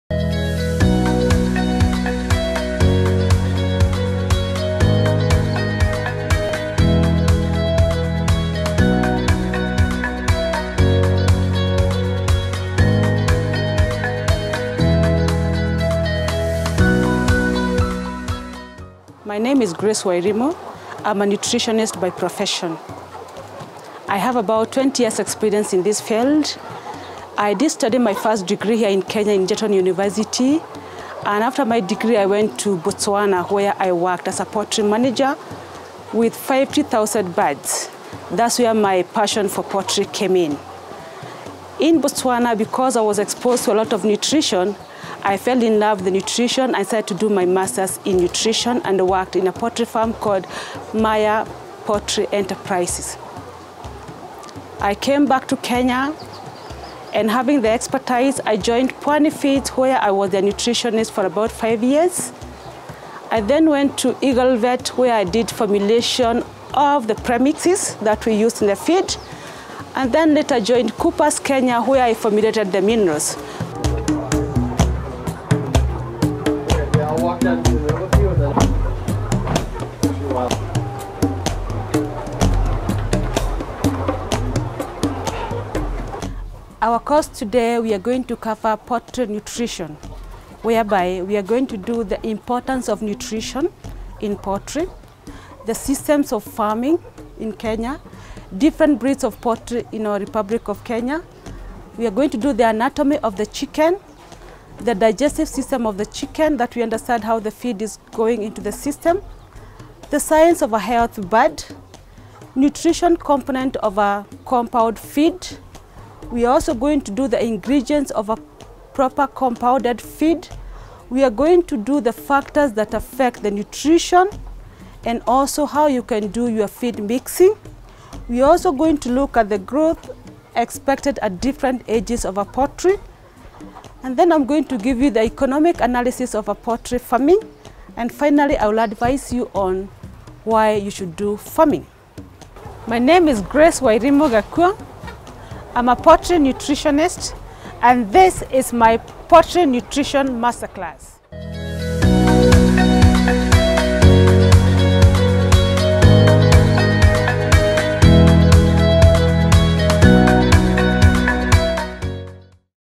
Poultry Nutrition (Lesson 1) - Introduction To Poultry Nutrition
Successful poultry production is dependent on three essentials - proper housing, management practices and adequate feeding/nutrition. To learn more, watch this video.

Want lifetime access to the entire course, a PDF manual, tests and a certificate for only USD 9.99?